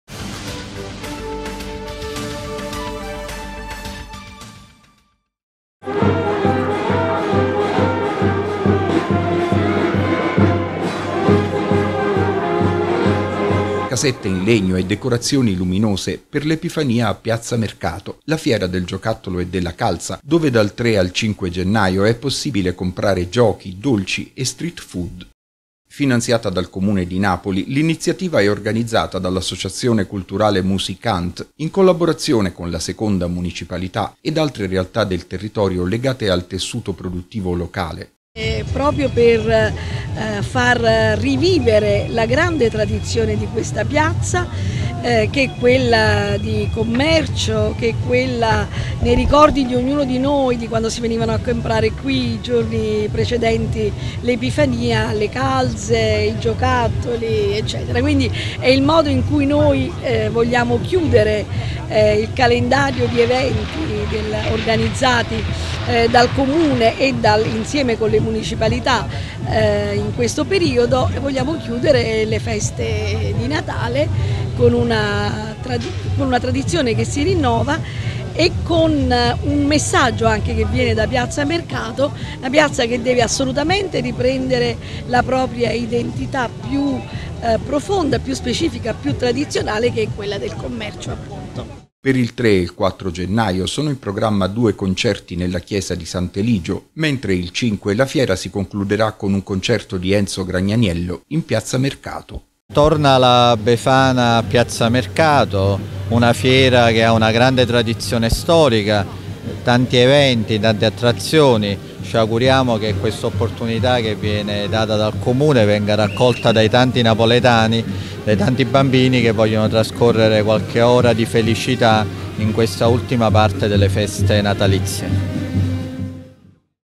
A piazza Mercato è cominciata la Fiera del giocattolo e della calza
Casette in legno e decorazioni luminose per «L’Epifania a piazza Mercato», la Fiera del giocattolo e della calza dove dal 3 al 5 gennaio è possibile comprare giochi, dolci e street food.
Per il 3 e 4 gennaio sono in programma due concerti nella chiesa di Sant’Eligio, mentre il 5 la fiera si concluderà con un concerto di Enzo Gragnaniello in piazza Mercato.